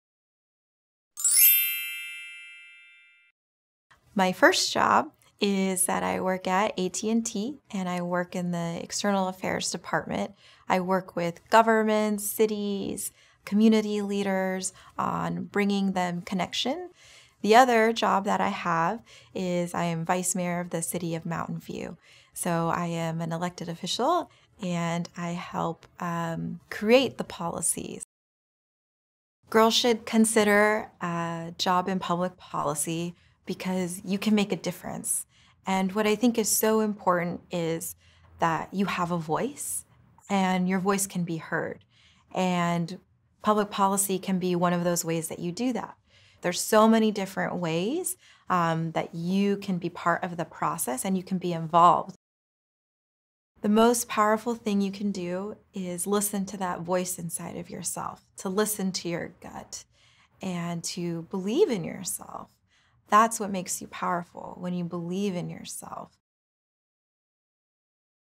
Vice Mayor | Ellen Kamei | 60 Seconds
Watch Ellen Kamei, External Affairs Area Manager with AT&T, and the Vice Mayor of the City of Mountain View, California.